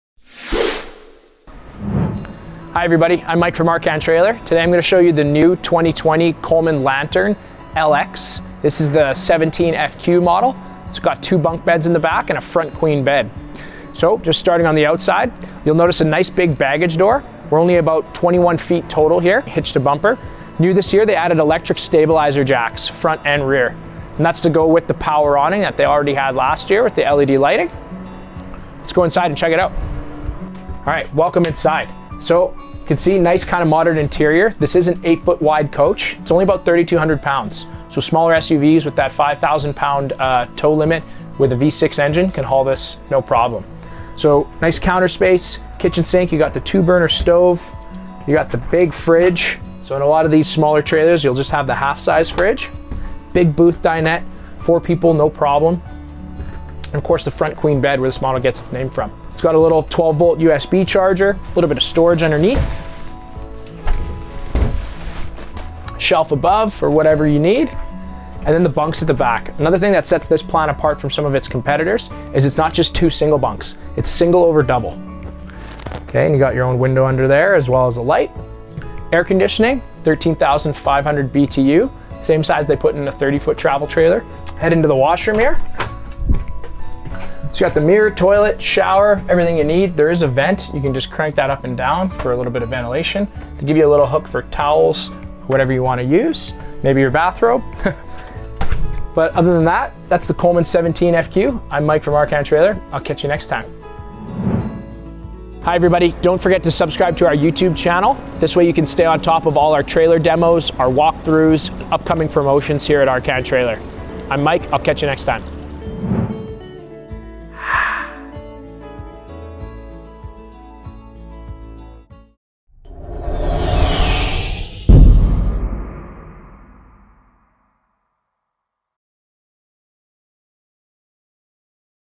2020 Dutchmen Coleman Lantern LT 17 FQ
Great sized family trailer that sleeps 4-6 people – park it on your driveway or in your back yard and it fits in any campground!  All the comforts you need for an enjoyable get-away...furnace, fridge, stove, microwave, foot pedal flush toilet, hot water tank, water heater bypass kit, rear camera prep, spare tire, ...and more!  Check it out today...
Coleman – the name you can Trust!